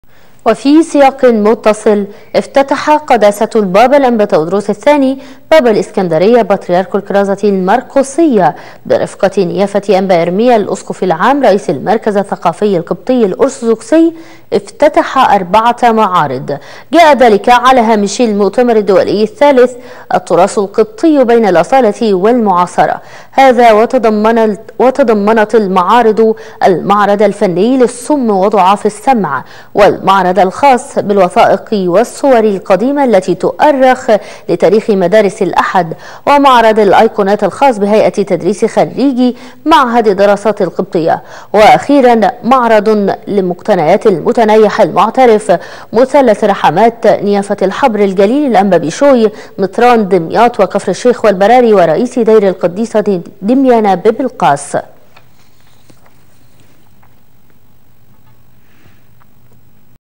بالفيديو // قداسة البابا تواضروس الثاني يفتتح ٤ معارض بالمركز الثقافي القبطي الأرثوذكسي
قداسة البابا تواضروس الثاني يفتتح ٤ معارض على هامش المؤتمر الدولي الثالث "التراث القبطيّ بين الأصالة والمعاصرة" بالتعاون بين المركز الثقافي القبطي الأرثوذكسي ومعهد الدراسات القبطية ..